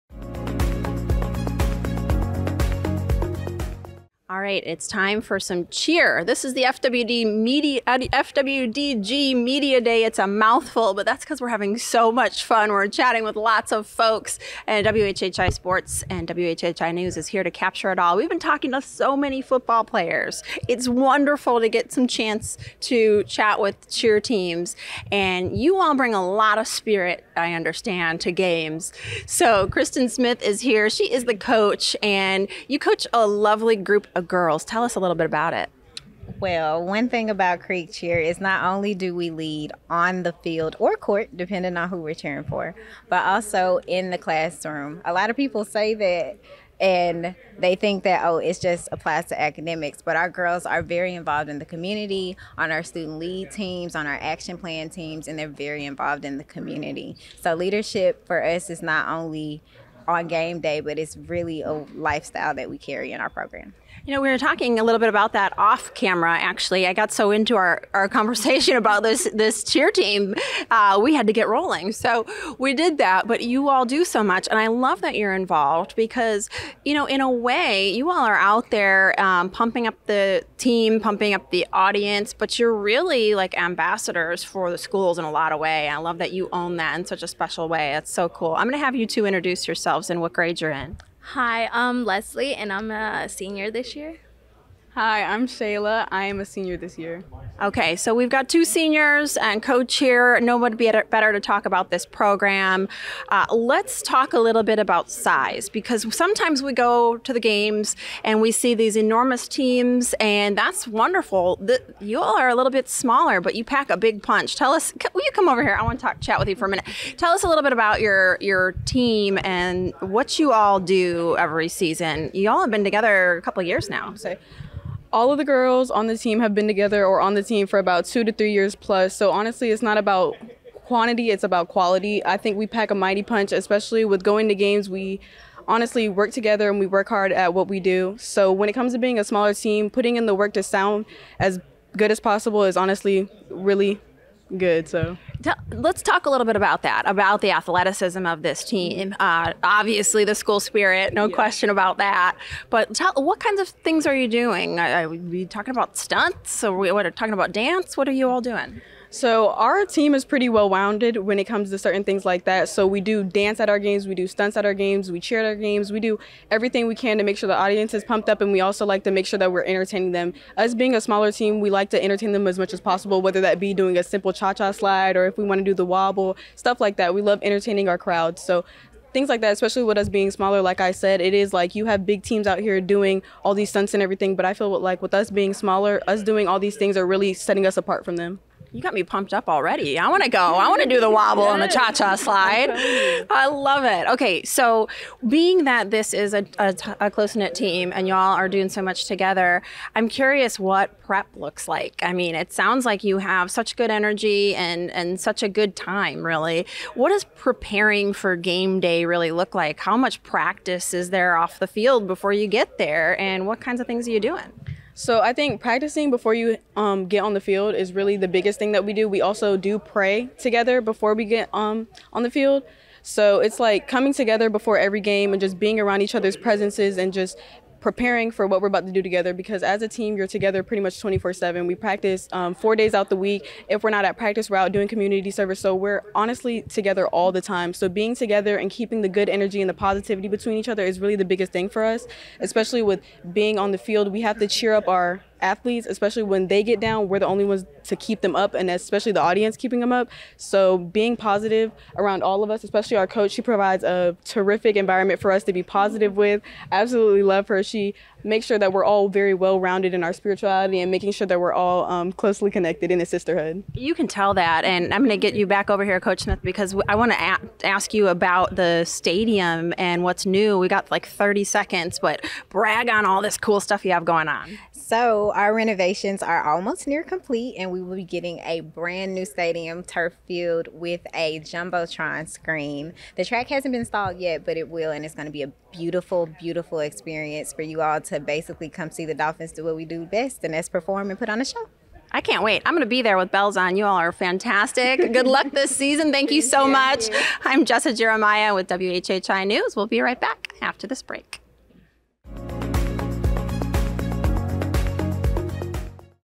WHHI NEWS | Battery Creek Dolphins Football Cheerleaders | Media Day 2022 | WHHITV Sports
The WHHI-TV High School Football Bowl Series
Presented by Summit Services Heating & Air

WHHI Media Day 2022 Filmed On Location at FWDG

FWDG:  Furniture Warehouse Design Gallery
745 Robert Smalls Pkwy, Beaufort, SC 29906

WHHI Daily News Anchor: Bob Stevens
On Location Reporter: Jessa Jeremiah

August 2022

32 Office Park Rd, Hilton Head Island, SC  29928